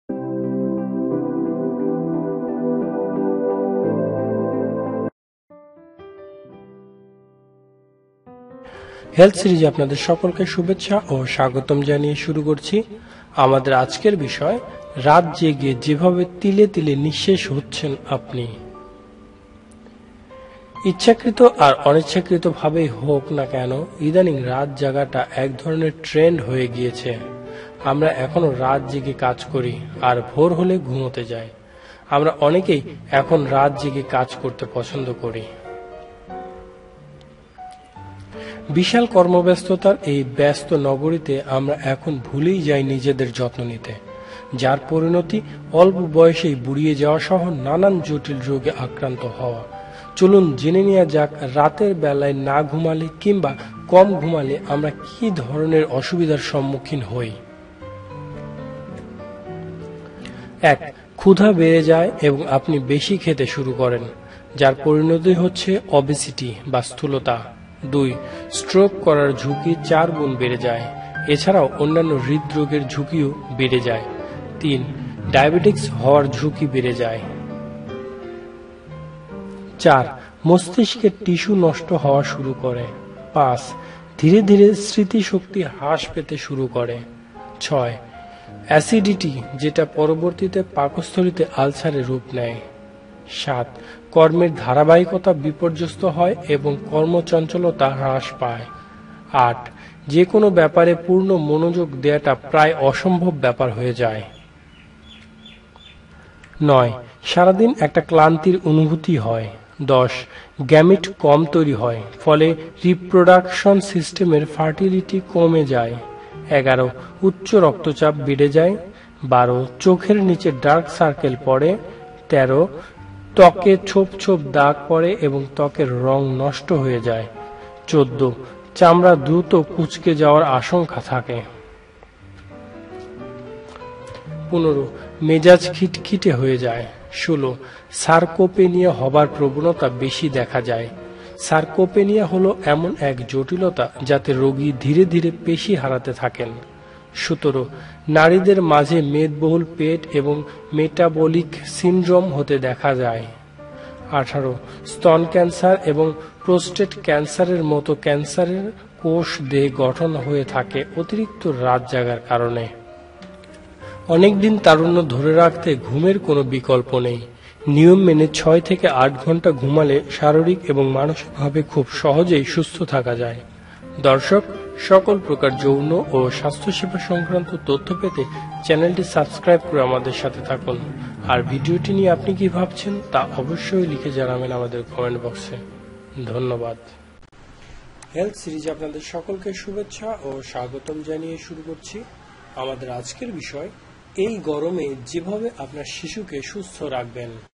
রাত জেগে যেভাবে তিলে তিলে শেষ হচ্ছেন আপনি। জানলে অবাক হবেন। By Health Series
ইচ্ছাকৃত আর অনিচ্ছাকৃতভাবেই হোক না কেন, ইদানিং রাত জাগাটা এক ধরনের ট্রেন্ড হয়ে গিয়েছে। আমরা এখন রাত জেগে কাজ করি আর ভোর হলে ঘুমাতে যাই। আমরা অনেকেই এখন রাত জেগে কাজ করতে পছন্দ করি। বিশাল কর্মব্যস্ততার এই ব্যস্ত নগরীতে আমরা এখন ভুলে যাই নিজেদের যত্ন নিতে । যার পরিণতি অল্প বয়সেই বুড়িয়ে যাওয়া সহ নানান জটিলরোগে আক্রান্ত হওয়া।
চলুন জেনে নেওয়া যাক রাতের বেলা না ঘুমালে কিংবা কম ঘুমালে আমরা কী ধরনের অসুবিধার সম্মুখীন হই।